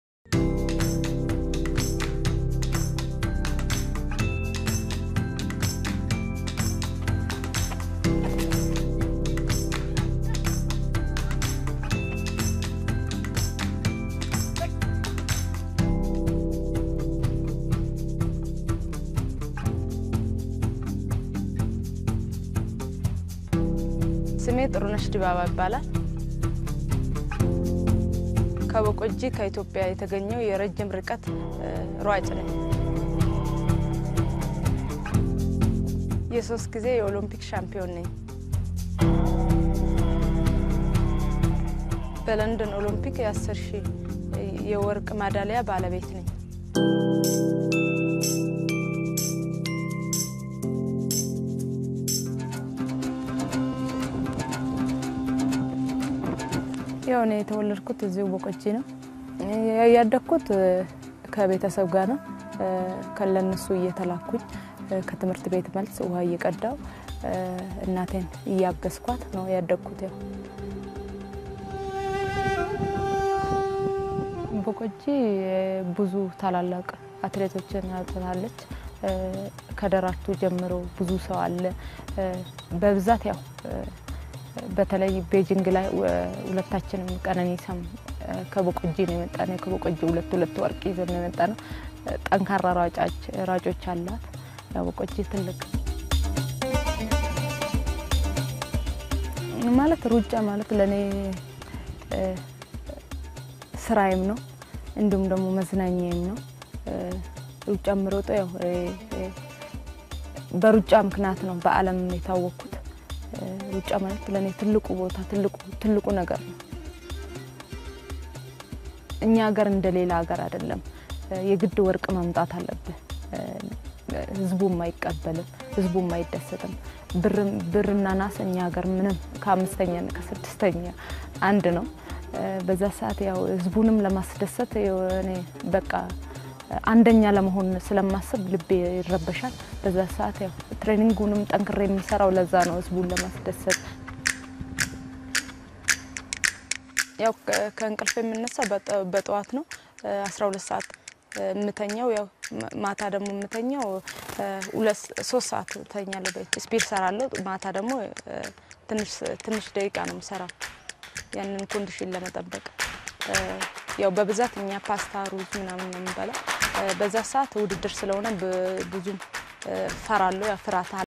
Ethiopia Sport Exclusive Interview With Olympic Gold medalist Tirunesh Dibaba-CNN | Must Watch...
Olympic Gold medalist Tirunesh Dibaba Win In 10000m